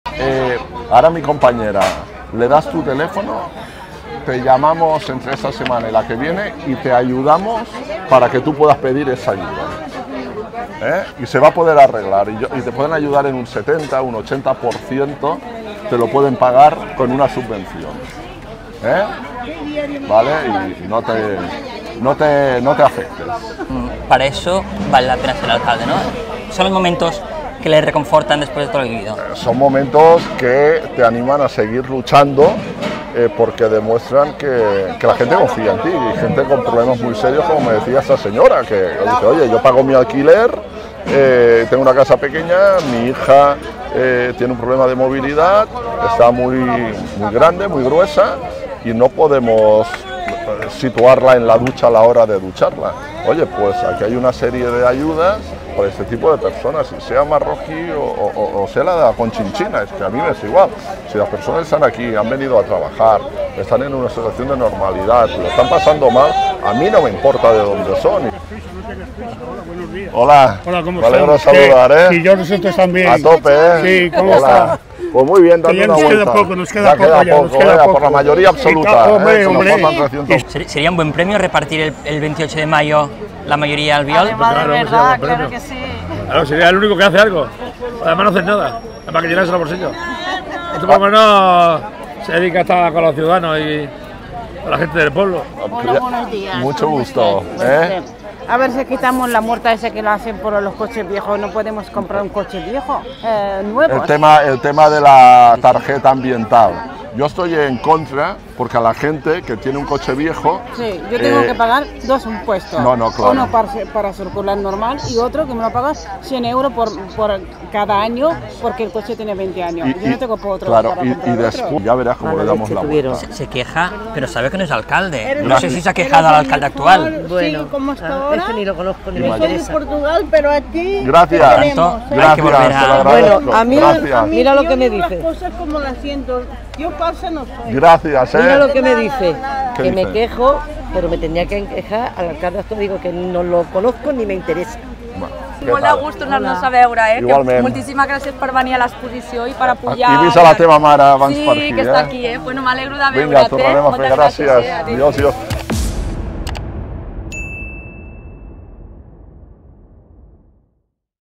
¡LA CALLE HABLA!: Los vecinos de Badalona a Albiol: «Queremos que vuelvas, hay que limpiar Badalona»
Xavier García Albiol es querido en Badalona. Tanto por los seguidores del PP como por «comunistas e independentistas» que, cuando se trata de las elecciones municipales, le prestan el voto a él. Así lo ha podido comprobar OKDIARIO durante la grabación de una entrevista por el mercado del barrio de la Salud. En poco más de 40 minutos de recorrido, que no se pudo completar por las dificultades de caminar, hasta 37 personas distintas pararon al candidato del PP para hacerle peticiones, agradecerle su trabajo o desearle suerte. «Hay que limpiar Badalona» le recordaba más de uno.